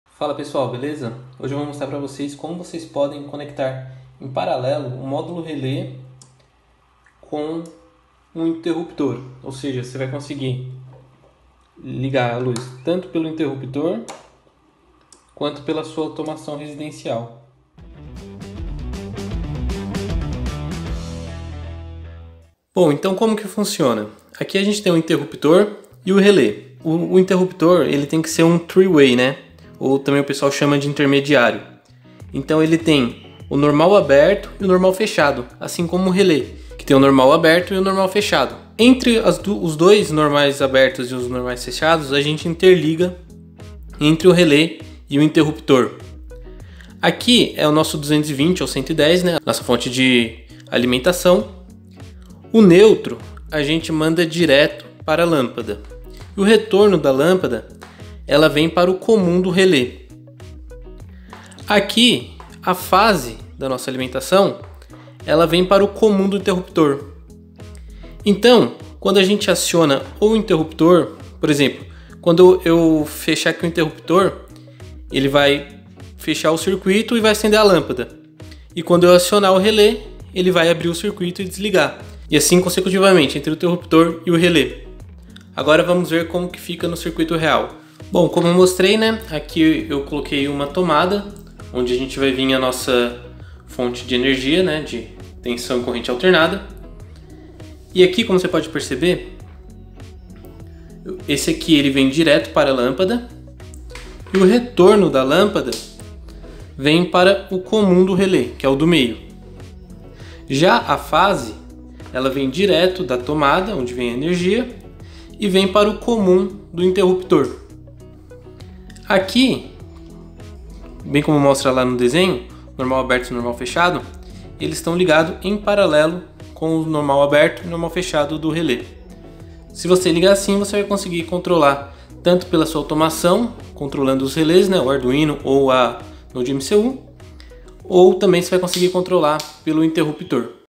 Como LIGAR a LUZ pelo CELULAR e pelo INTERRUPTOR
Neste vídeo eu mostro como você pode ligar a sua automação junto com o interruptor e controlar a luz pelo celular ou fisicamente.

Quer aprender a criar suas automações residenciais? Conheça nosso treinamento completo